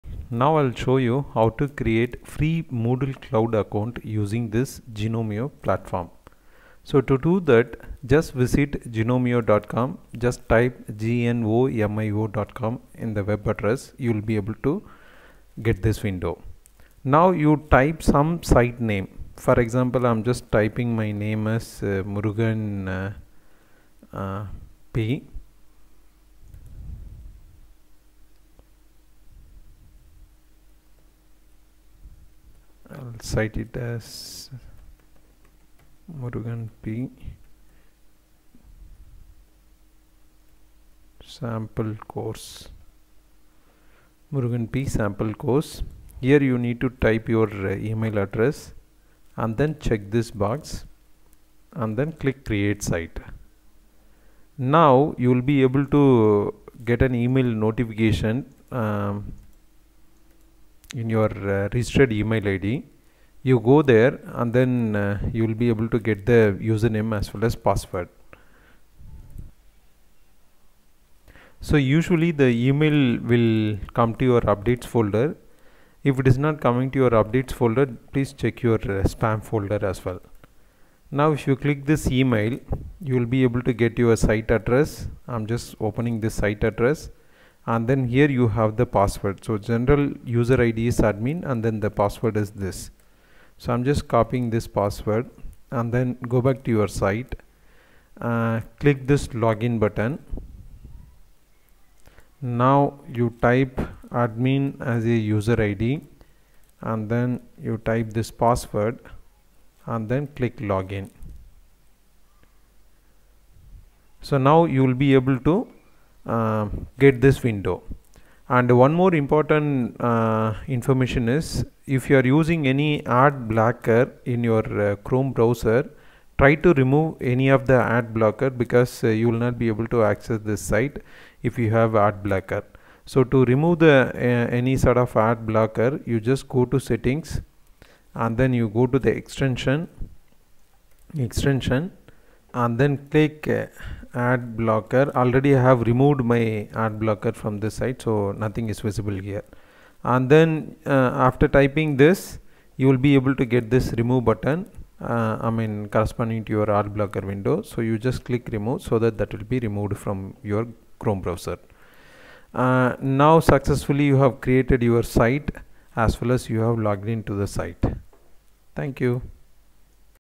Creating gnomio site address
This video demonstrates the process of creating gnomio moodle site. Gnomio is a free cloud platform to use Moodle software.